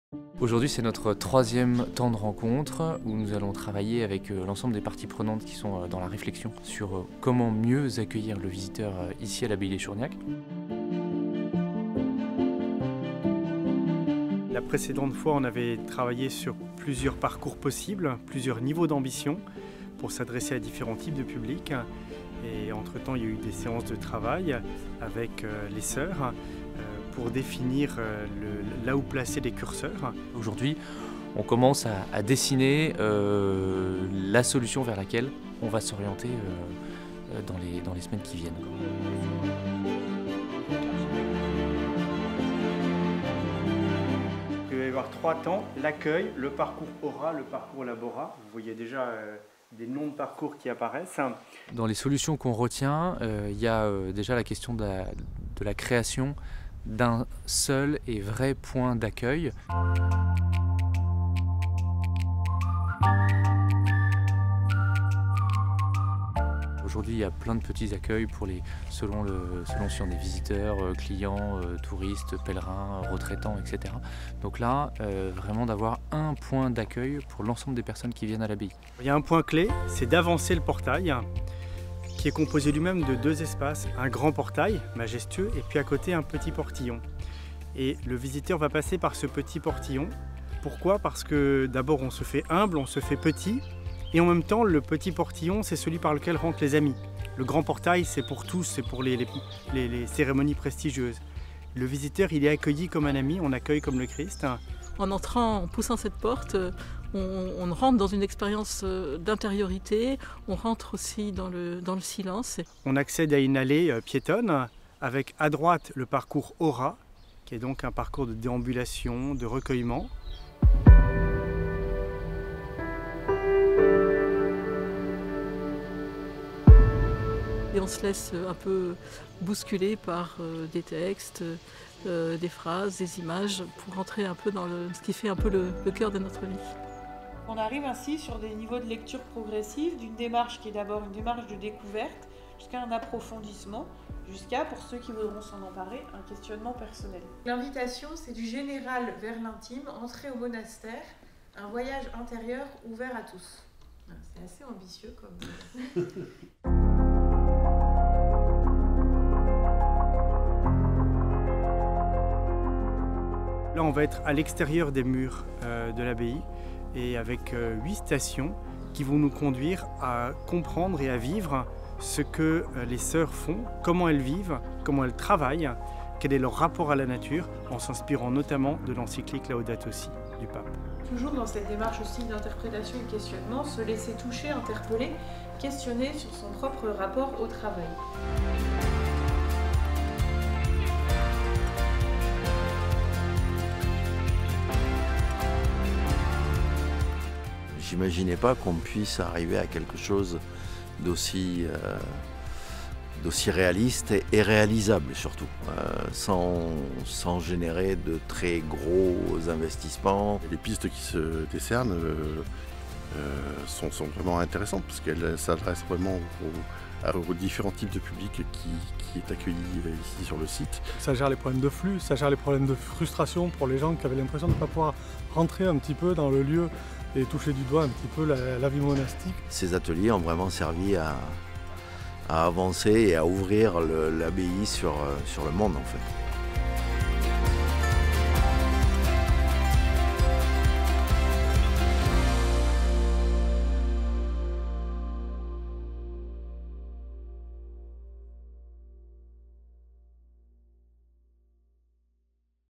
Création d'un parcours de visite : 3ème et dernier atelier collectif
Le troisième et dernier atelier de travail collaboratif autour de l’amélioration de l’accueil des visiteurs de l’abbaye d’Echourgnac s’est achevé dans la satisfaction. Les visiteurs pourront bientôt faire l’expérience de ce qui se vit dans les murs de l’abbaye grâce à deux parcours de visite, et un nouvel accueil unifié.